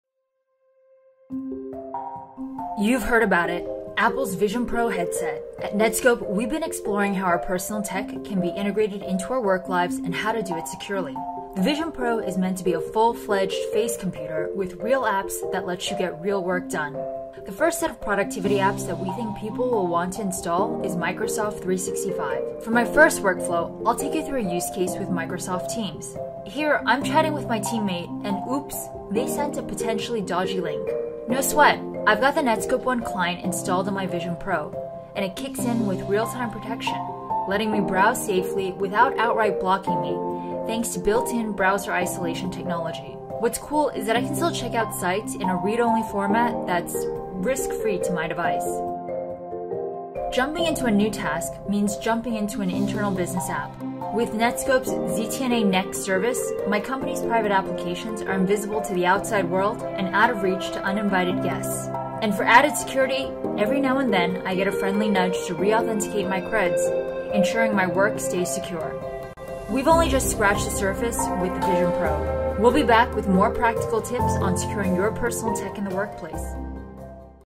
How to Use Netskope Security with the Apple Vision Pro
Can the VR headsets make the leap from a cool gadget to a serious productivity tool for businesses?

We got our hands on the Apple Vision Pro headset and installed the Netskope Client to explore how our customers can  unlock its productivity benefits without sacrificing security.

In this demo video, we’ll show you two scenarios:
Safe collaboration and browsing: Collaborate seamlessly on Microsoft Teams with built-in protection from risky links thanks to Netskope Remote Browser Isolation.
Secure private applications: Access your company’s internal applications safely with Netskope ZTNA Next.